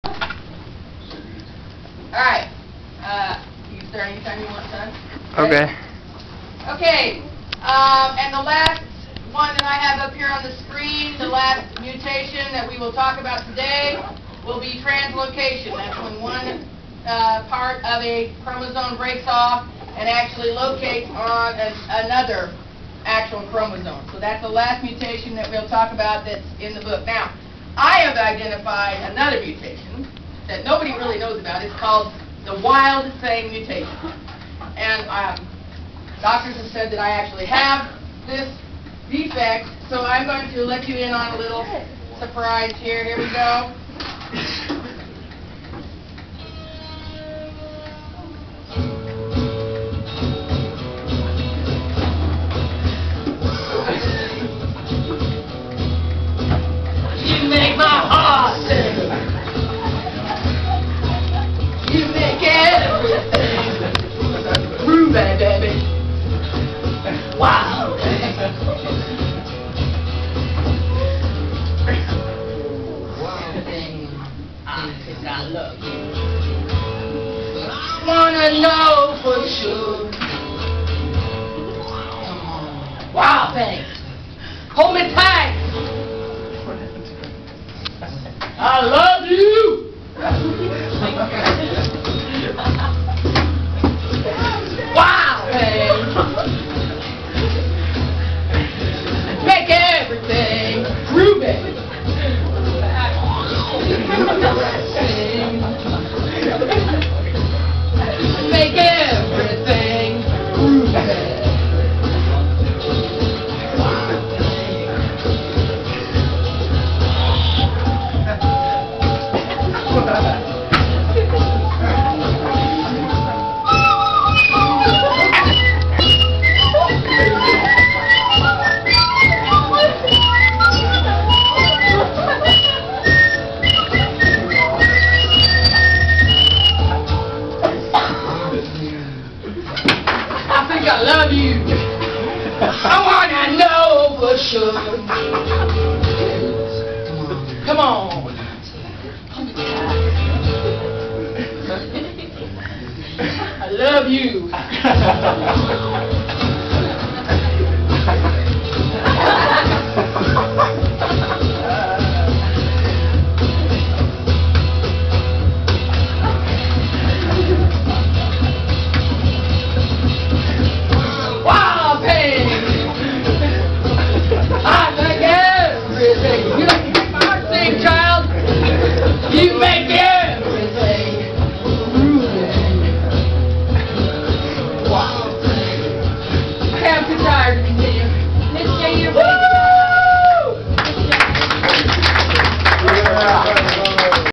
Wild Thang
Our high school biology teacher, Ms. J., shows off her genetic mutation in front of her first period class.